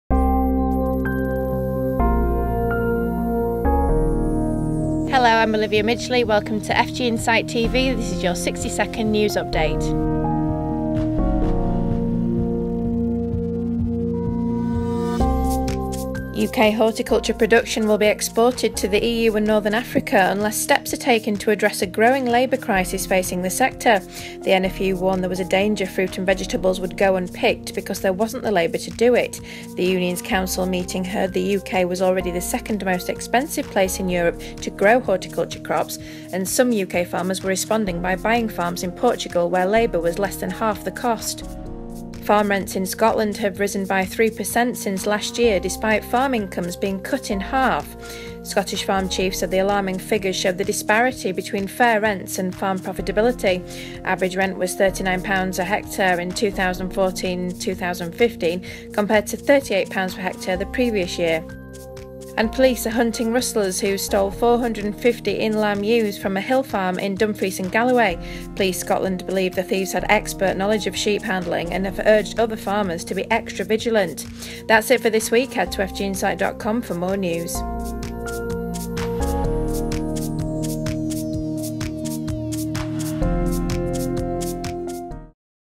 FGinsight 60 second news: April 21, 2016
The NFU have warned there could be a danger of fruit and vegetables being unpicked due to a growing labour crisis. Meanwhile farm rents in Scotland have seen a three percent increase since last year, and finally 450 ewes have been stolen from a farm in Dumfries and Galloway.